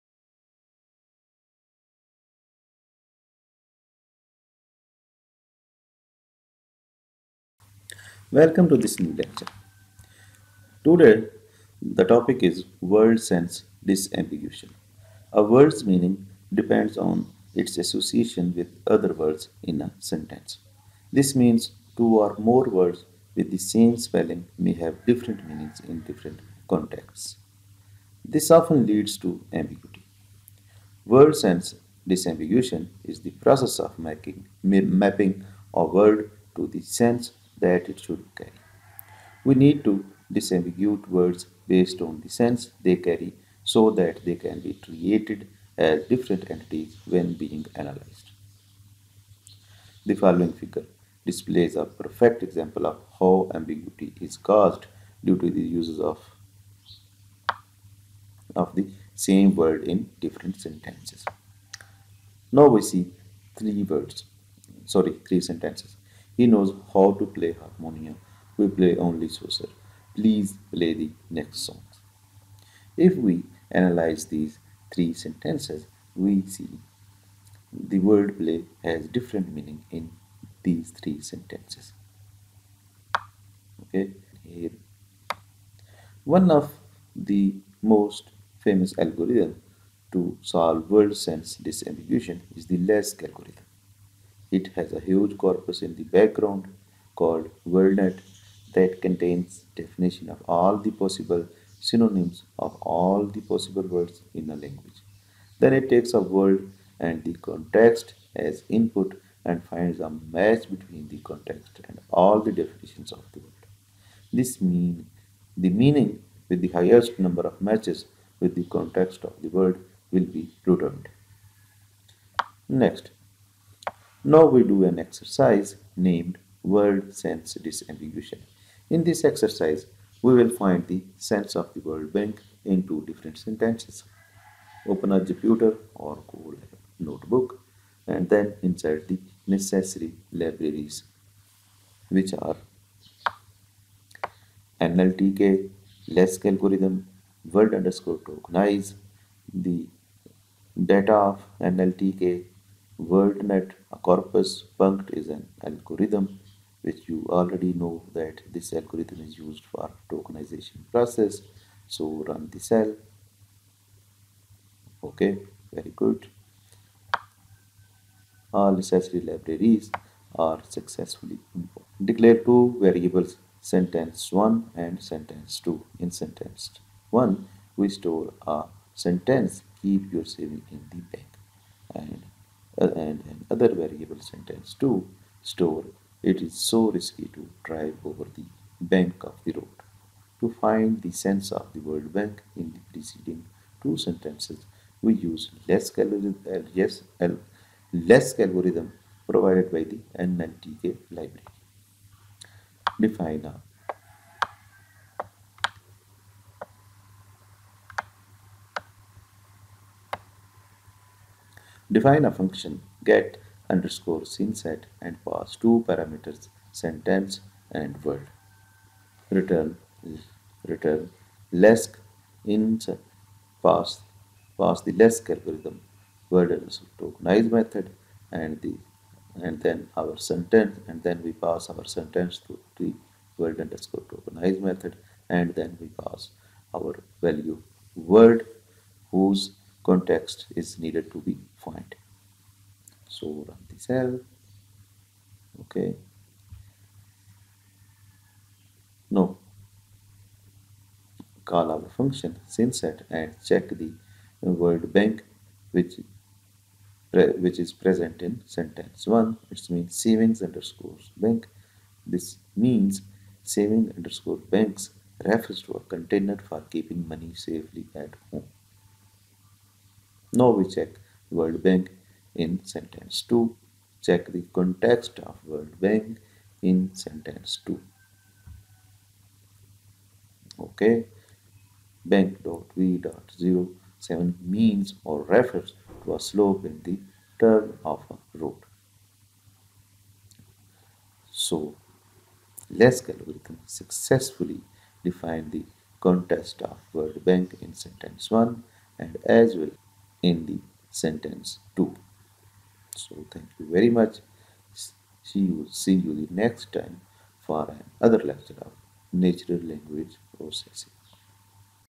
Word Sense Disambiguation
A word's meaning depends on its association with other words in a sentence. This means two or more words with the same spelling may have different meanings in different contexts. This often leads to ambiguity.
Word sense disambiguation is the process of mapping a word to the sense that it should carry.